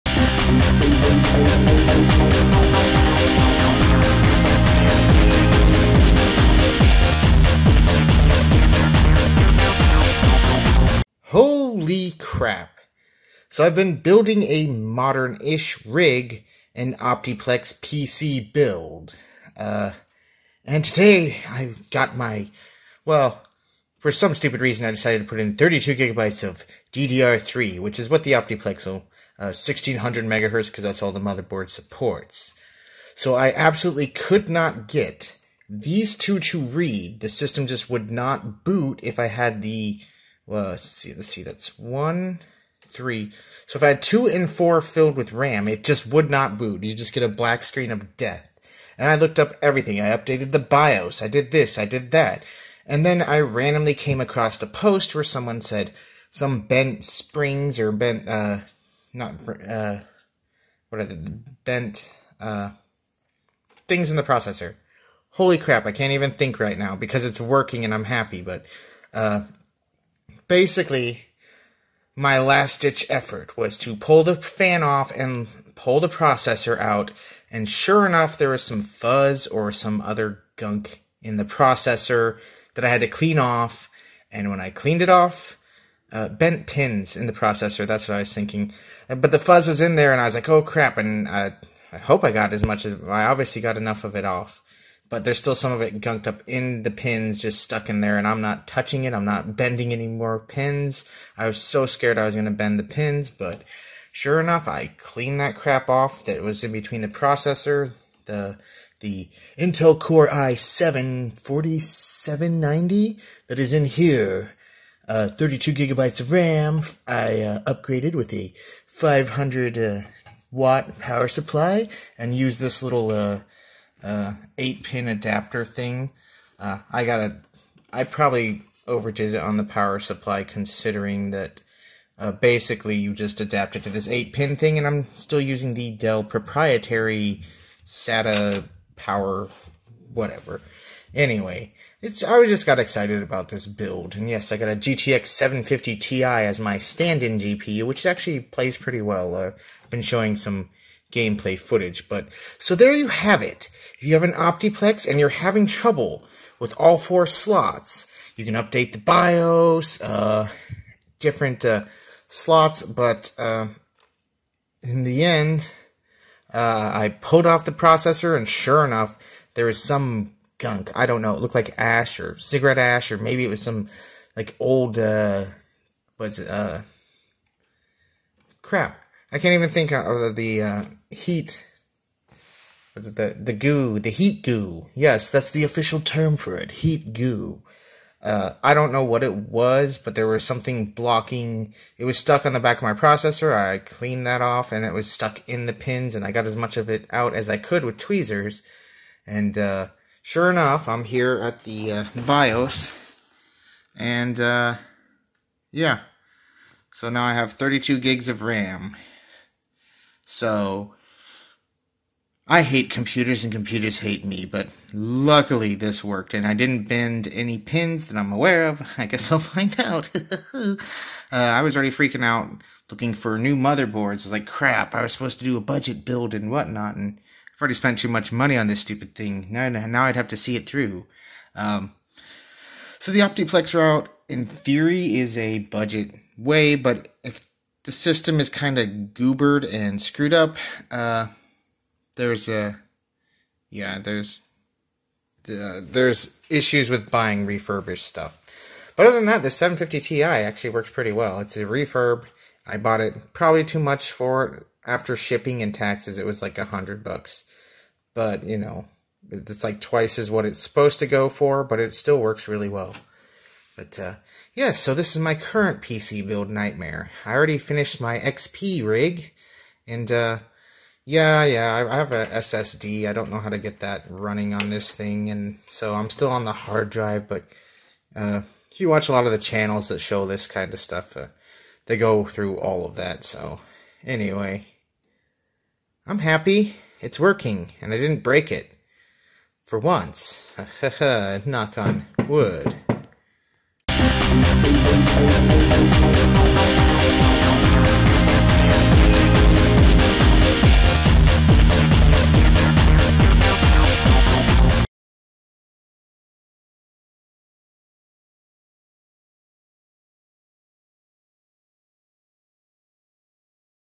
Troubleshooting DIMM Slots 2 and 4 Optiplex 9020
Just Finished my Windows XP PC build, and now I am working on upgrading my modern editing/some gaming rig. I went the Optiplex upgrade route for a budget build. This is a refurbished Optiplex I just got in a couple of weeks ago, and today, while trying to put in the 32 Gigs of DDR3 (1600 mhz) which is the max ram, and max speed this thing supports, I couldn't get DIMM slots 2 and 4 to work. In fact, the PC would just boot to a black screen. If I just left the ram in the first two slots (DIMM 1 and 3) it would work fine.

I googled it, and went through the troubleshooting process as suggested, and nothing worked. I finally came across a post about how bent pins could be the cause, so I, finally, took the processor out and inspected it just to find there was gunk between the processor and the pins, and it had been there for a while. It even had an outline on the processor itself. Just goes to show, if you buy a refurbished Optiplex, make sure you check the processor as your beginning maintenance just to make sure.

The case is also bent, but that could have happened in shipping. From now on though, any future rigs I use will be built by me.